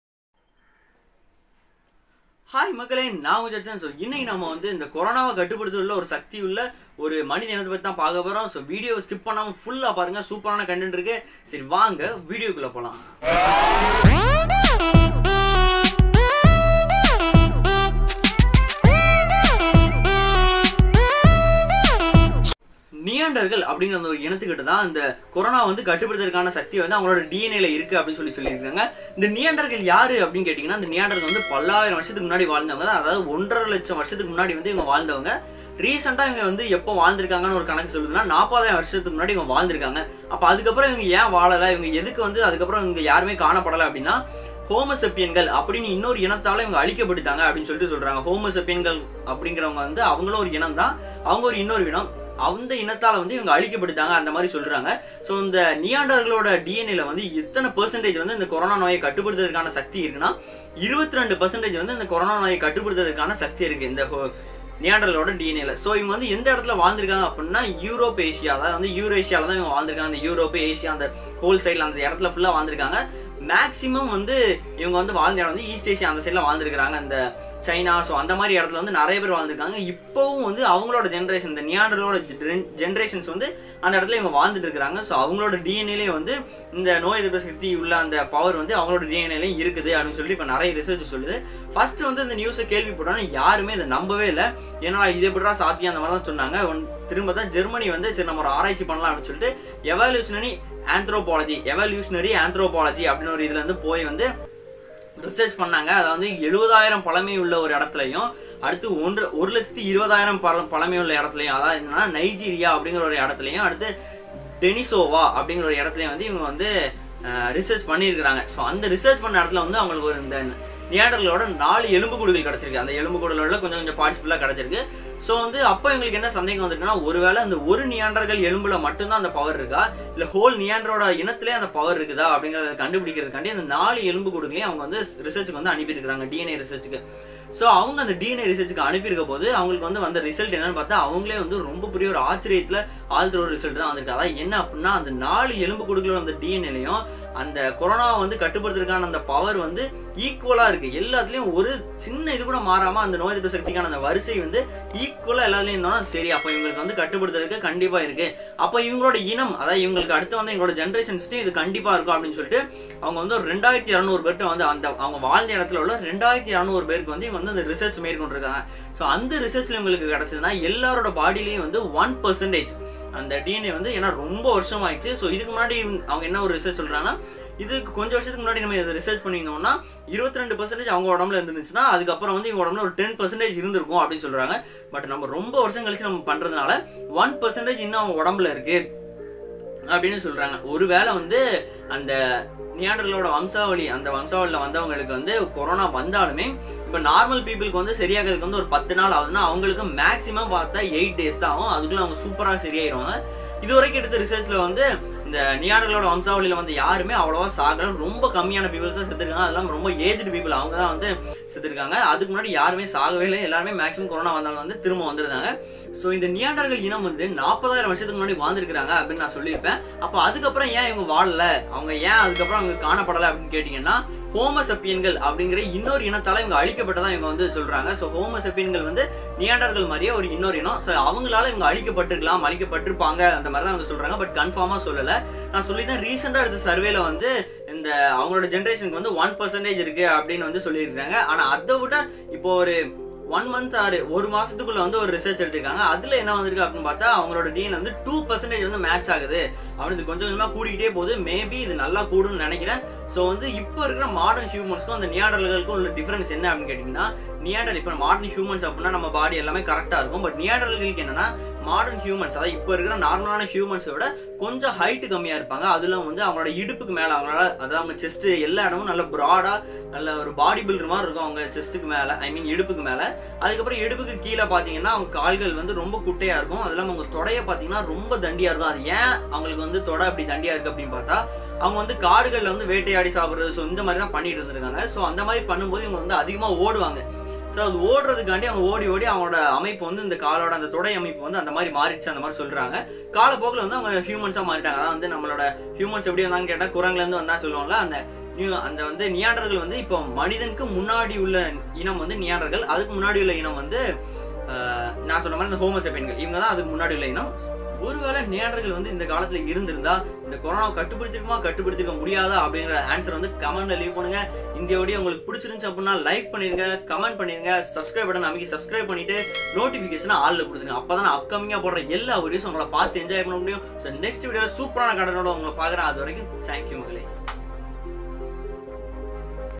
A man have the power to control கொரோனா|possible?|who is the man?|explained in Tamil.
An ancient group of people has the power to control Corona. They have the power in their DNA. I explained it in Tamil.